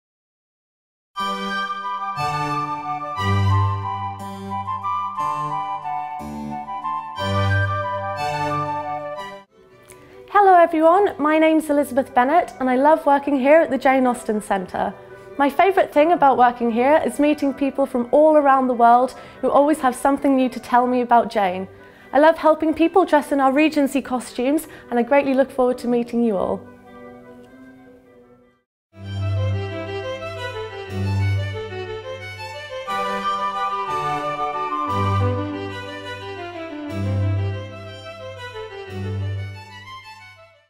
Meet the Staff - 'Elizabeth Bennet'
'Elizabeth Bennet', one of the staff at the Jane Austen Centre talks about at the Centre in Bath